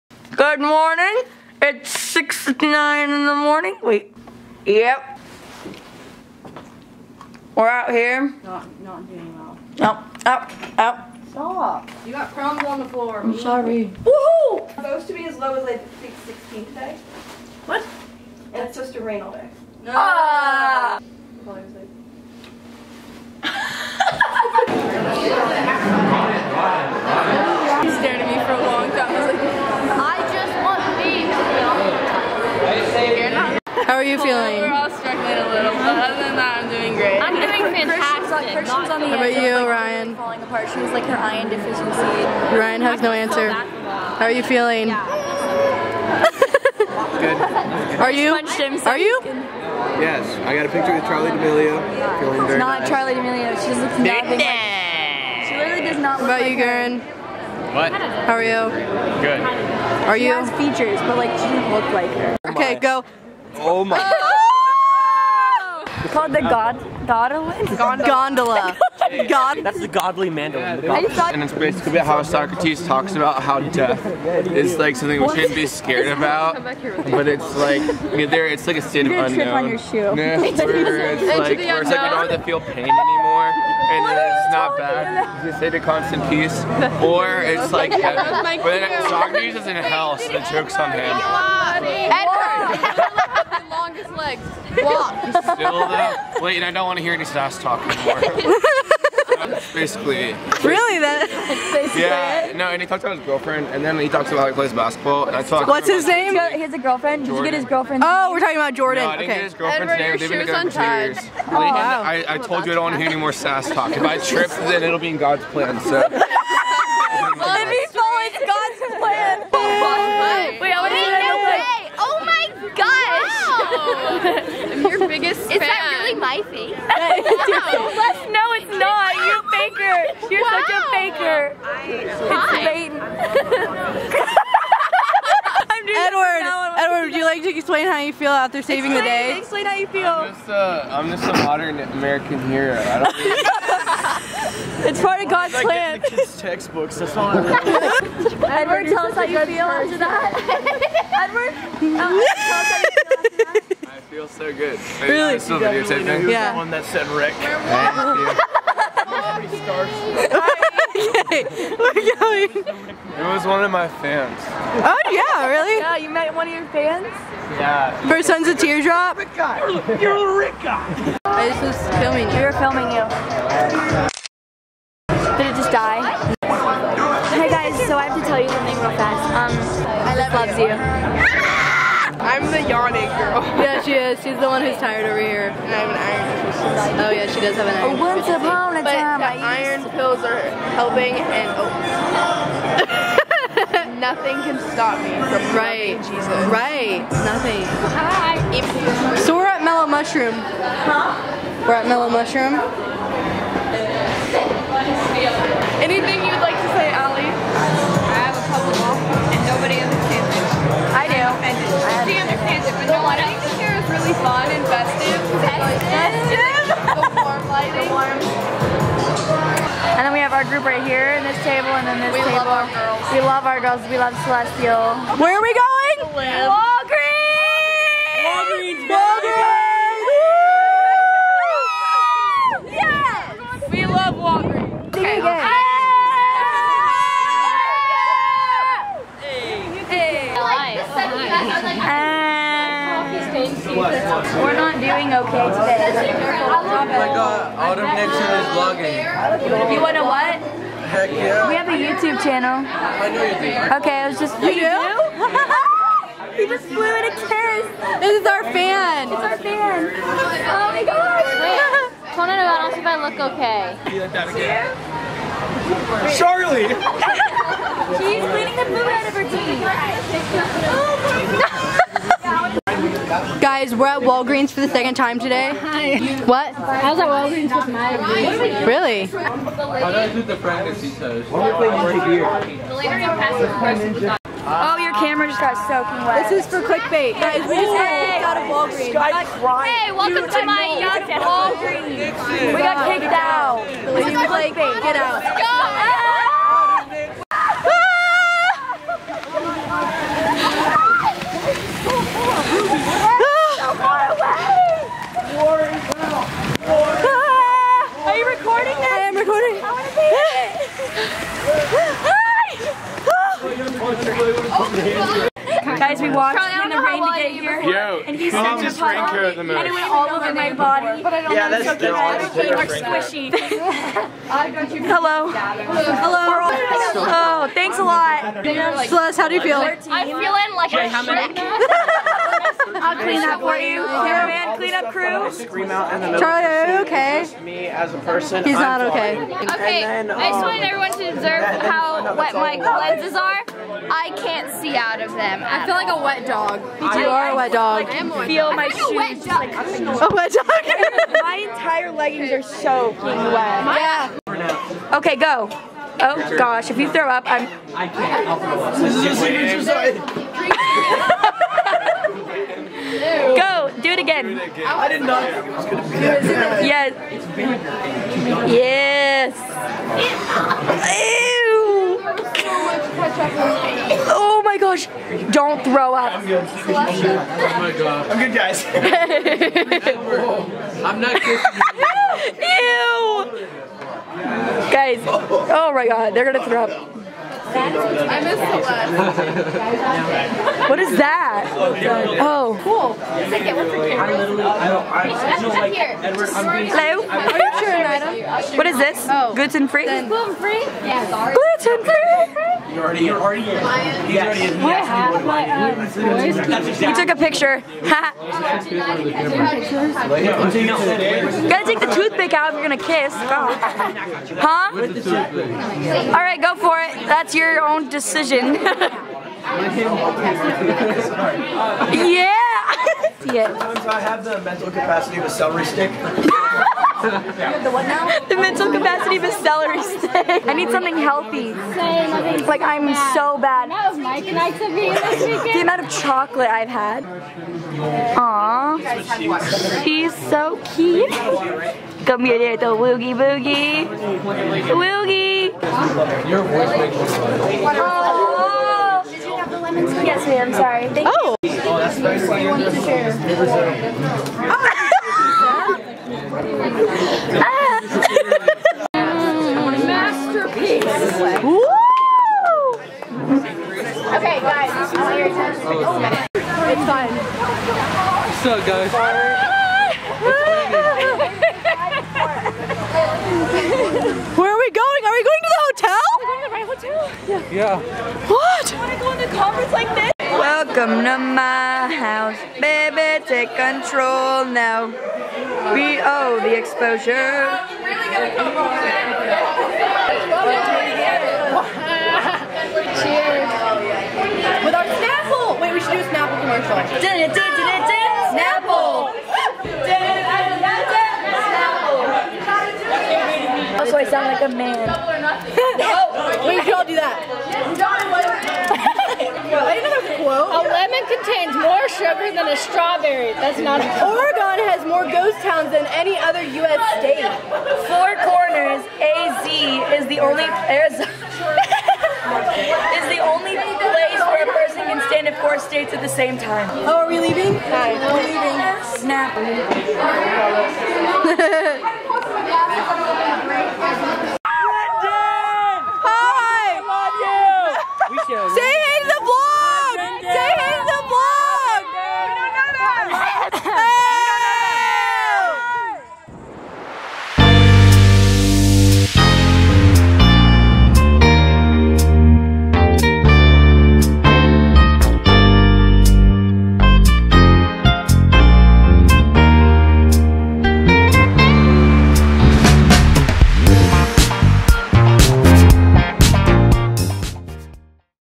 Crossroads Winter Conference Jan. 2020 // Day 2 vlog
Sorry everyone for taking an entire year and a half to getting around to editing and posting the rest of the Crossroads vlogs. Very fun times. None of us look like these people in the video because it was over a year ago. hilarioussssss